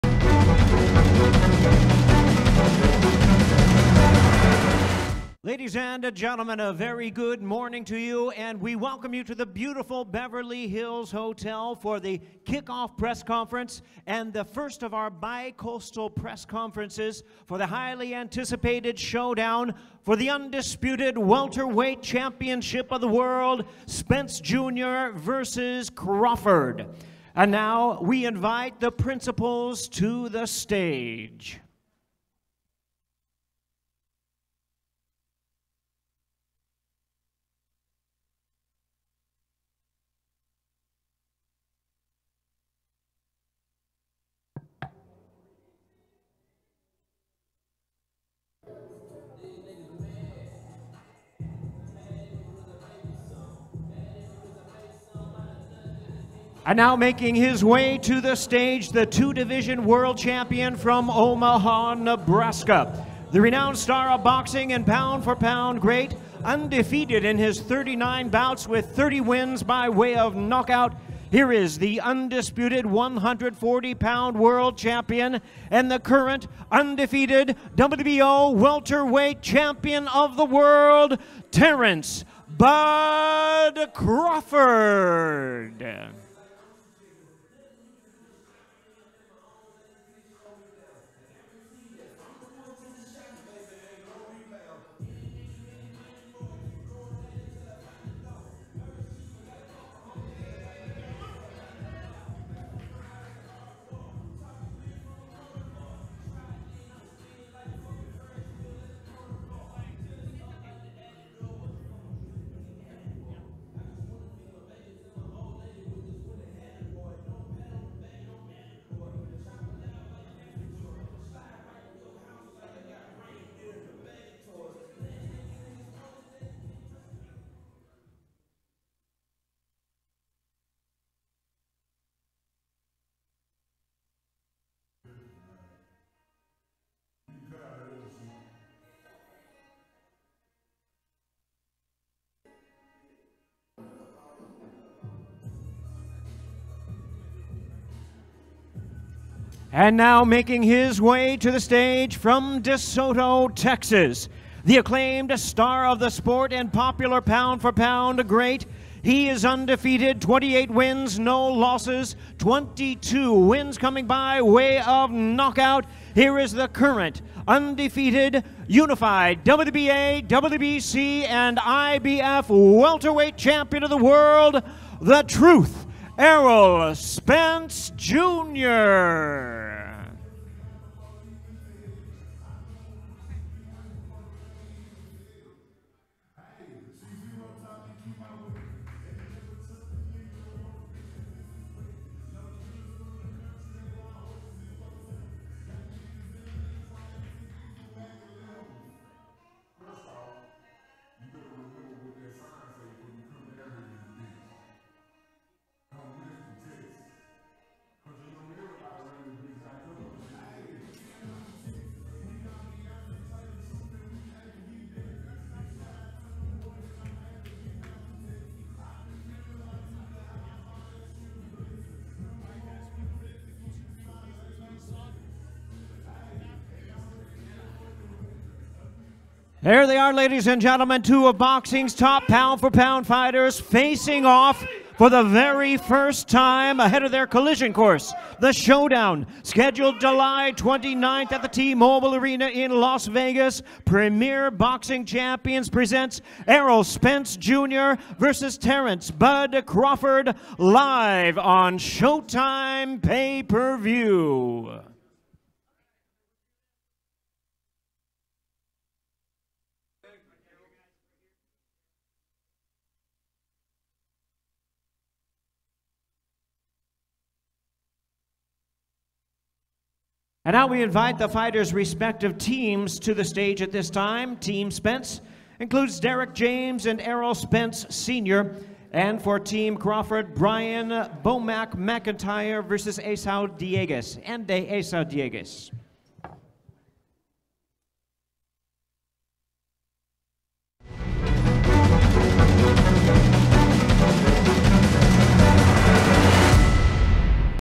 FIRST FACE OFF | Errol Spence vs. Terence Crawford • HEAD TO HEAD in Beverly Hills | PBC Boxing
LOS ANGELES | FIRST FACE OFF | Errol Spence vs. Terence Crawford • HEAD TO HEAD in Beverly Hills | PBC Boxing

Undefeated Top Pound-For-Pound Stars Errol Spence Jr. and Terence Crawford Meet for Undisputed Welterweight World Championship Saturday, July 29 Headlining a Premier Boxing Champions Event on SHOWTIME PPV from T-Mobile Arena in Las Vegas

Unified WBC, WBA and IBF Welterweight World Champion Errol “The Truth’’ Spenceand WBO 147-pound World Champion Terence “Bud’’ Crawford will go coast-to-coast for a pair of press conferences in Los Angeles and New York as they square off for the first time to preview their undisputed welterweight championship showdown taking place Saturday, July 29 headlining a SHOWTIME PPV from T-Mobile Arena in Las Vegas in an event presented by Premier Boxing Champions.

Two of the top pound-for-pound fighters in the sport, Spence and Crawford will now finally meet to crown the first undisputed welterweight champion of the four-belt era and add their name to the long list of stars that have conquered boxing’s glamour division.